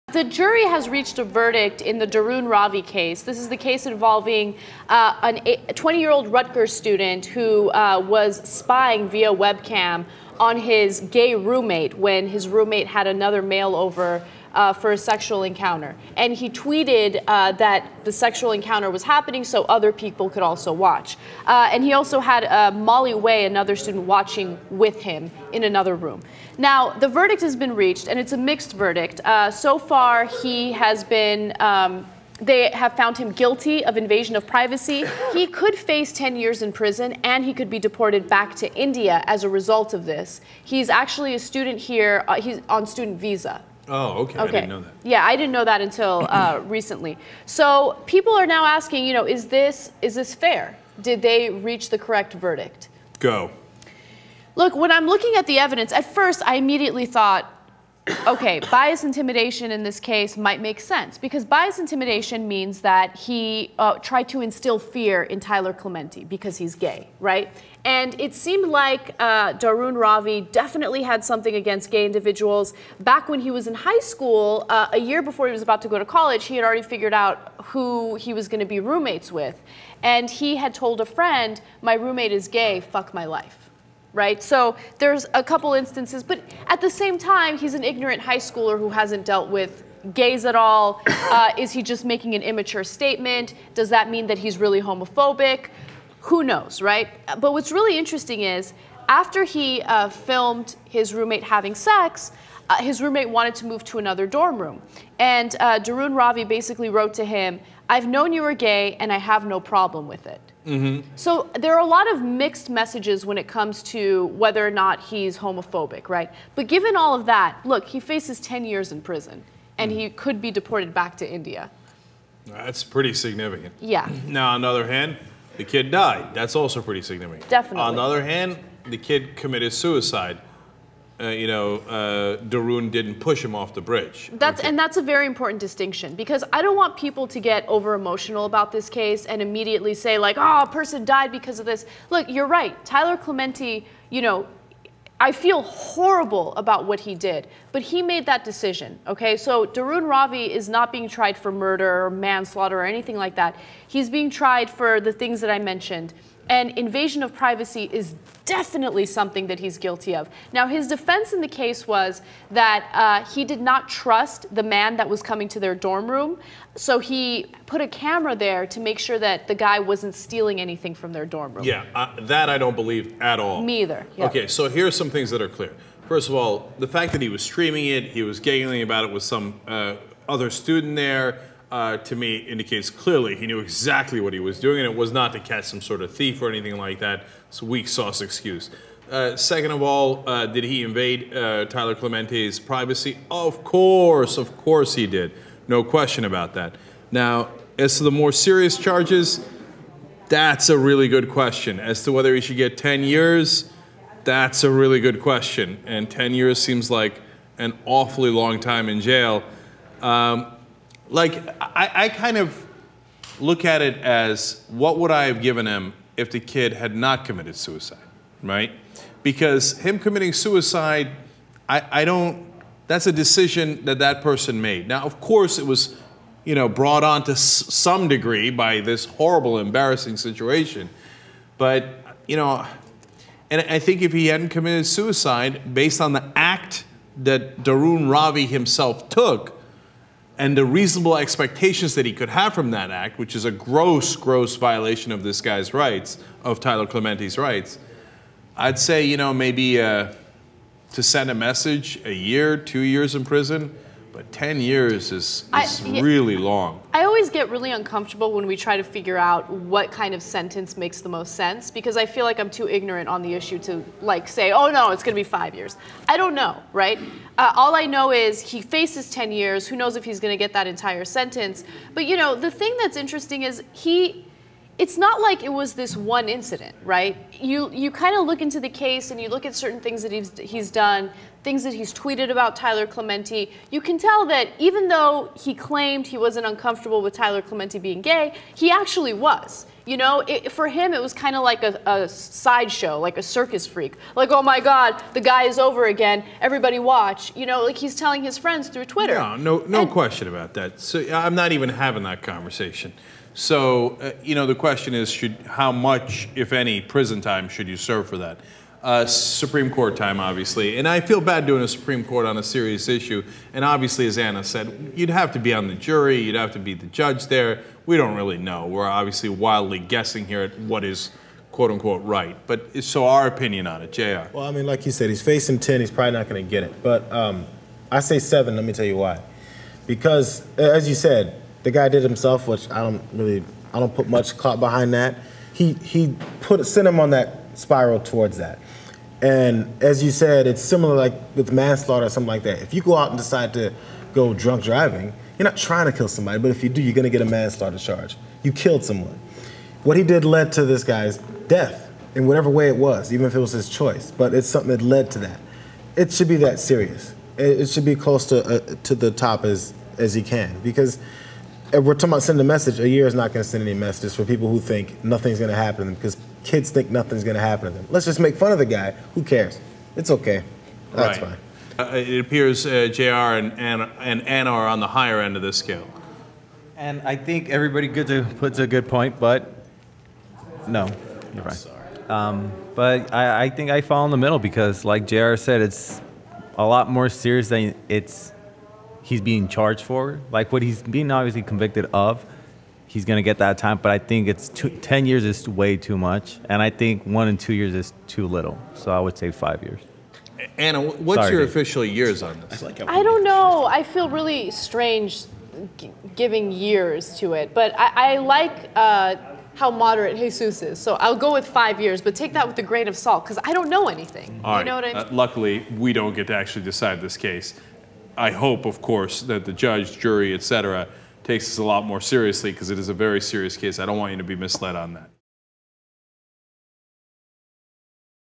Jury Verdict In Rutgers Suicide Case
A jury convicted Rutgers University student Dharun Ravi guilty of of hate crimes for using a webcam to spy on his roommate, Tyler Clementi kissing another man in their dorm room. The case became famous largely because Tyler Clementi committed suicide shortly after being filmed by Dharun Ravi.